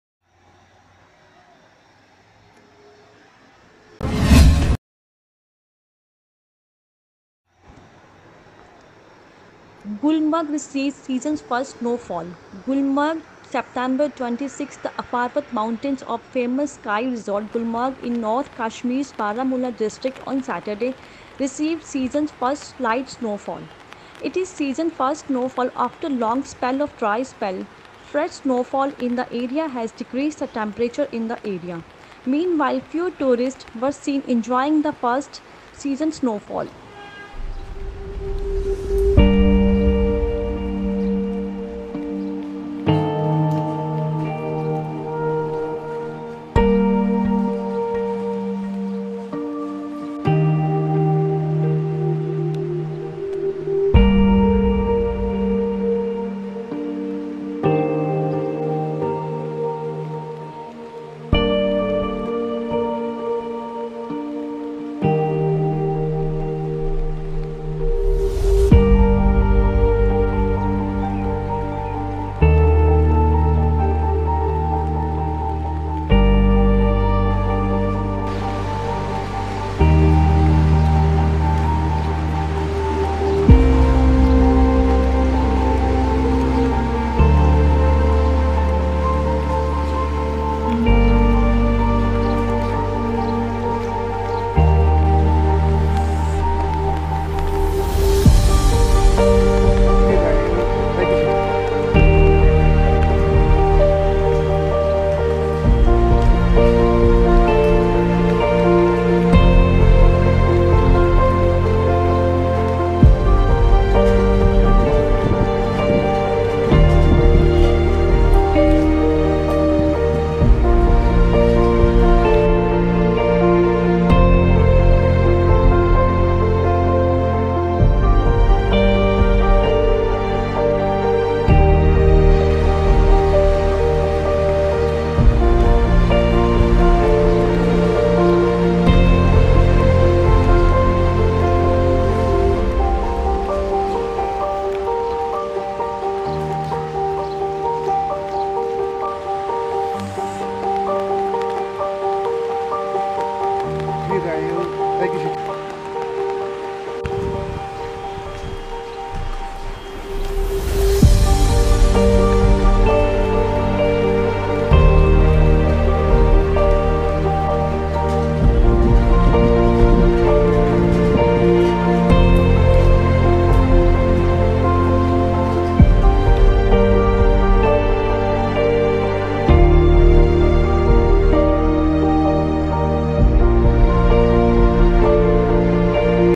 Gulmarg receives seasons first snow fall on Saturday
Gulmarg receives seasons first snow fall

Gulmarg, Sep 26: The Affarwat Mountains of famous Ski resort Gulamarg in North kashmir’s Baramulla district on Saturday received seasons first light snow fall.

It is seasons first snowfall after long spell of dry spell.Fresh snowfall in the area has decreased the temperature in thea area.

Meanwhile, few tourist were seen enjoying the season’s first snowfall